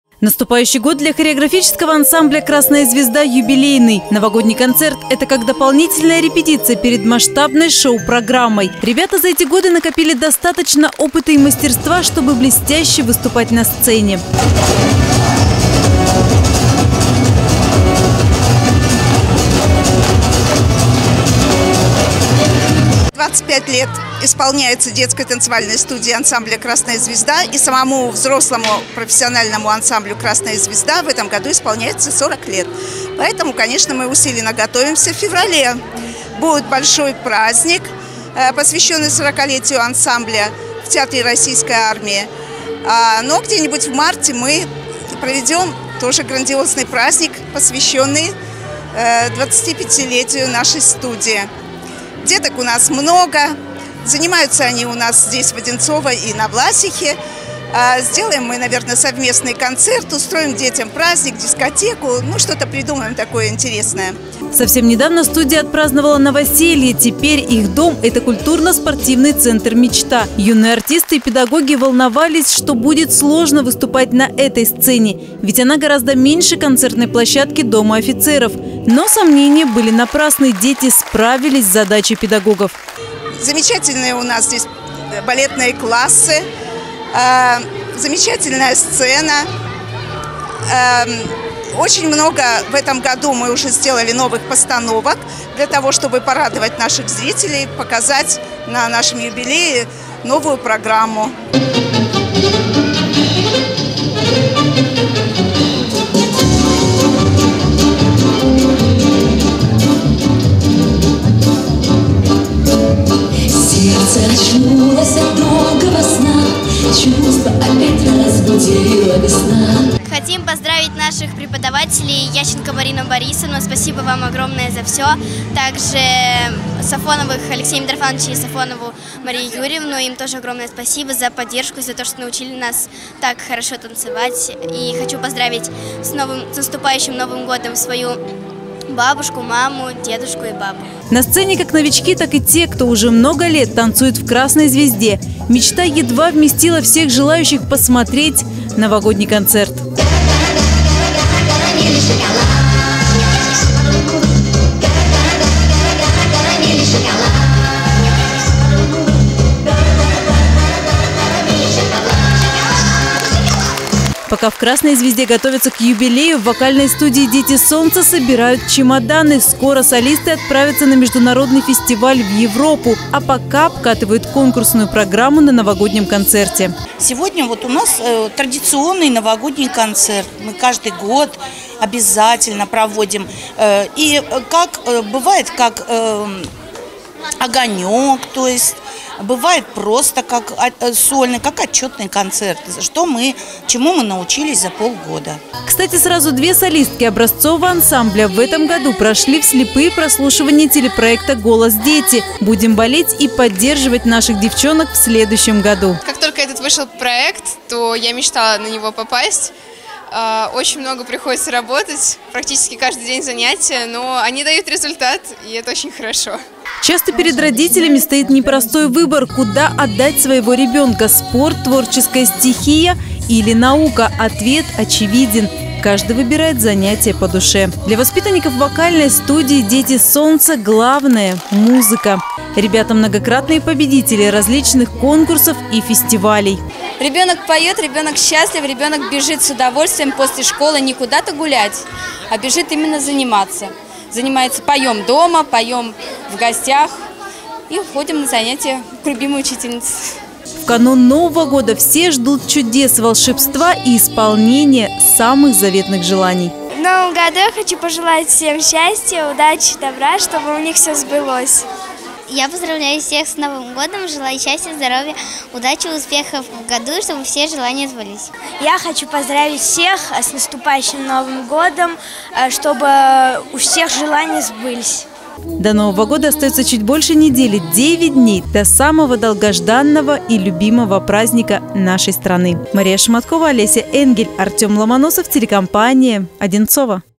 Отчетные концерты в детских коллективах
Традиционно в канун Нового года во всех студиях, ансамблях и кружках проходят отчетные концерты. Главные действующие лица – дети. Они показывают все, чему научились за полгода. Все сценические площадки задействованы под проведение спектаклей и развлекательных программ. Нашей съемочной группе удалось побывать сразу на двух новогодних концертах.